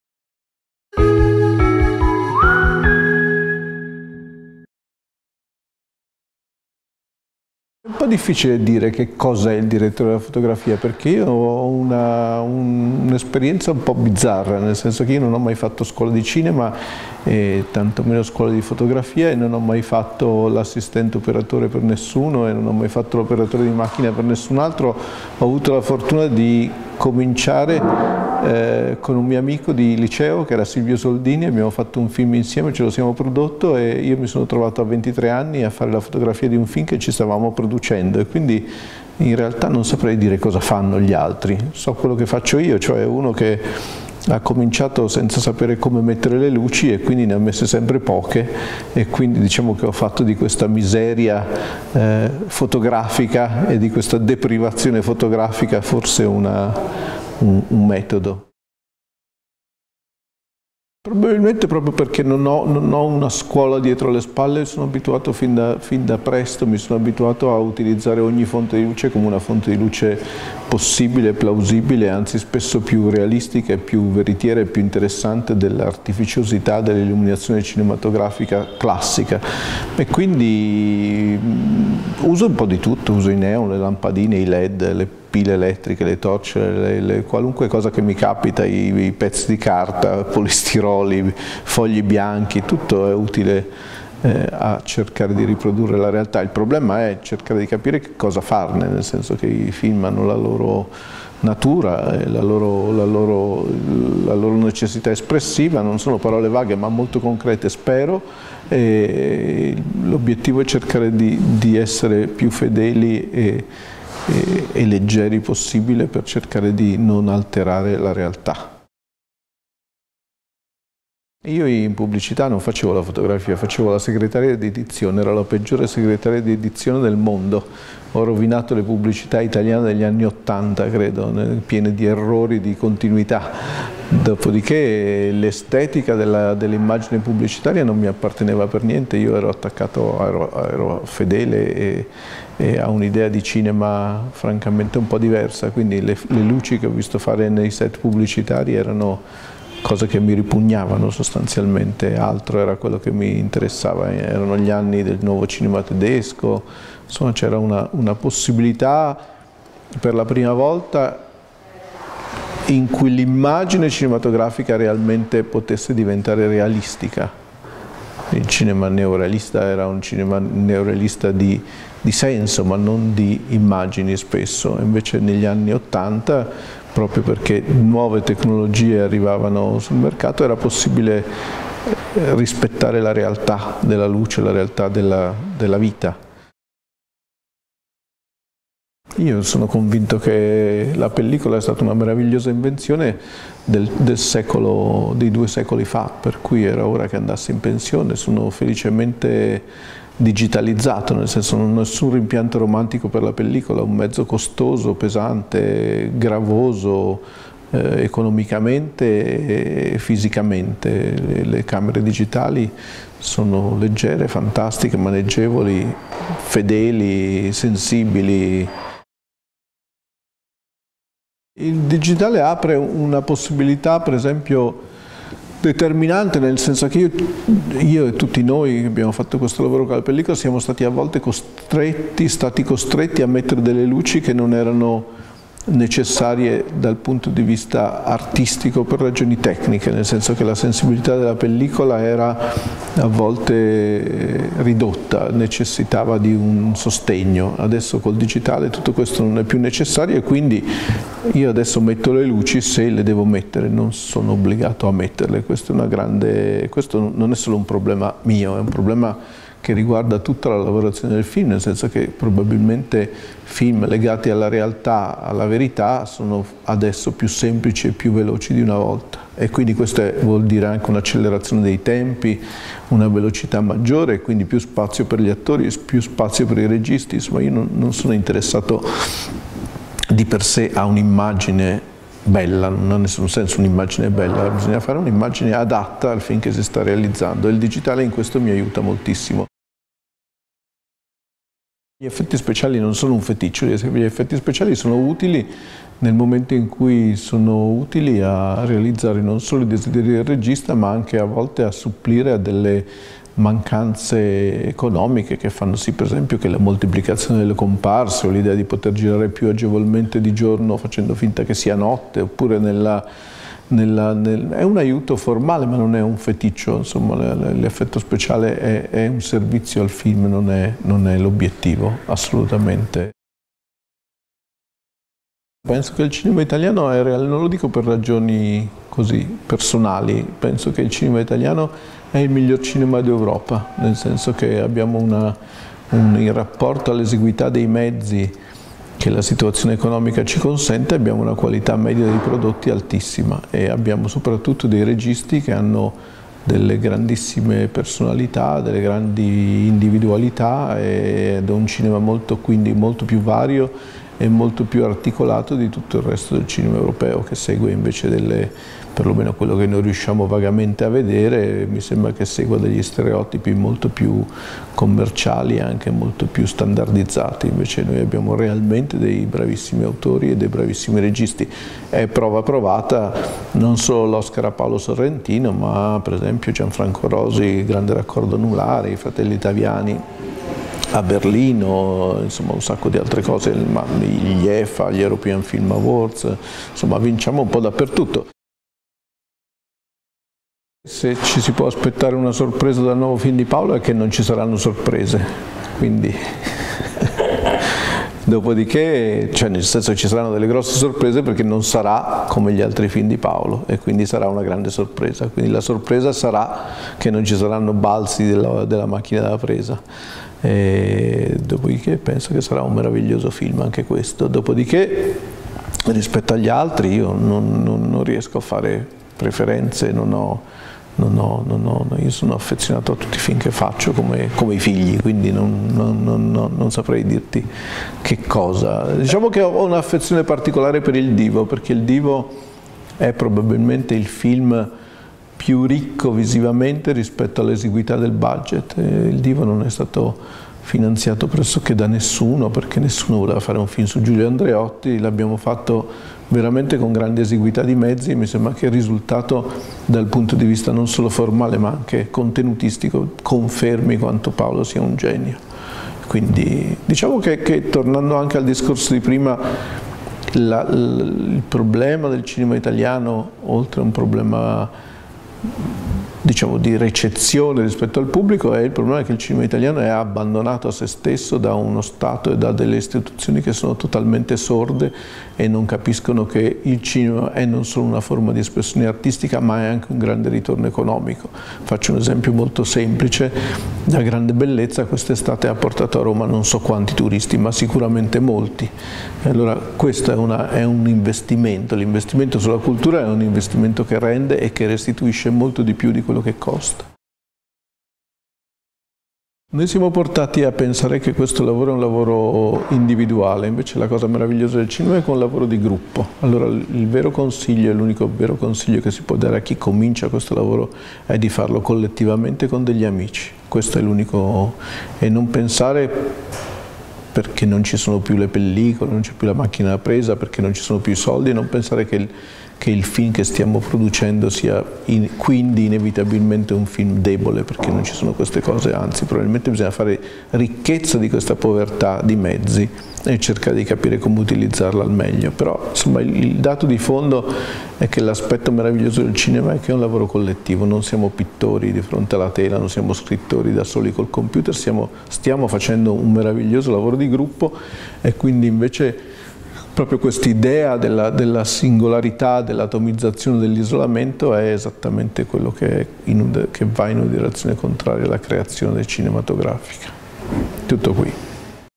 Luca Bigazzi: "Il cinema italiano è il migliore d'Europa"
A lezione di cinema con uno dei più importanti direttori della fotografia italiani, Luca Bigazzi, che ci racconta trent'anni di carriera e di successi firmati dietro la macchina da presa al fianco di grandi autori del nostro cinema: da Gianni Amelio fino al sodalizio con Paolo Sorrentino.